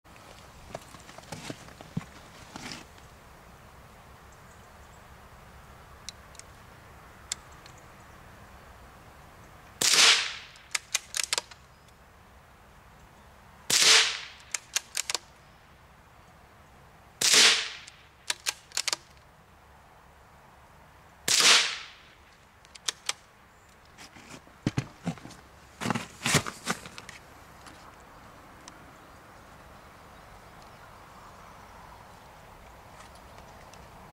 .17HMR Shooting Accuracy - 35 yds
Four rounds put through the same hole at 35 yds. Gun is Anchutz 1517 with factory shortened barrel. Mag only holds four rounds.